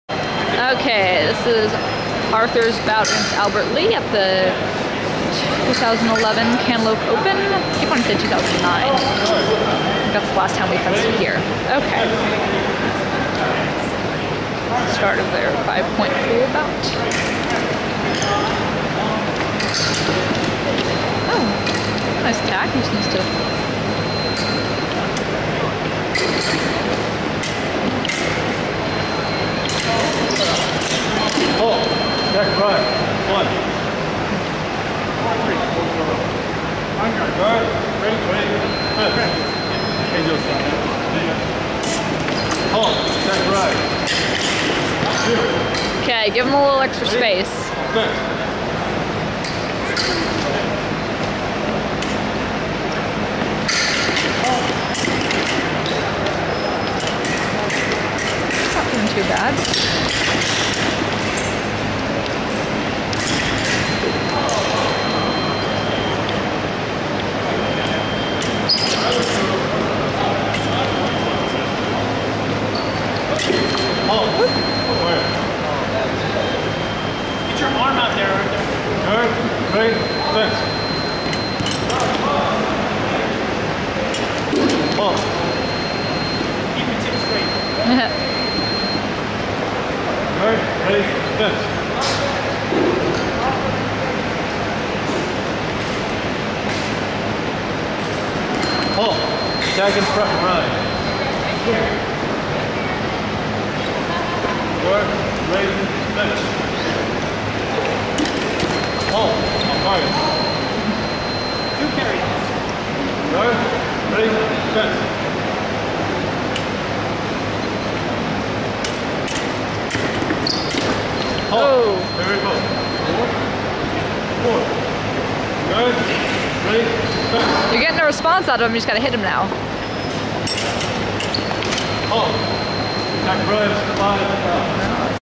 2011 Cantaloupe Open - Foil pools - Cheng v. Lee
2011 Cantaloupe Open - Open Mixed Foil pools - Arthur Cheng (ISUFC) v. Albert Lee. Forker Building, Iowa State University, Ames, IA. Oct. 8, 2011.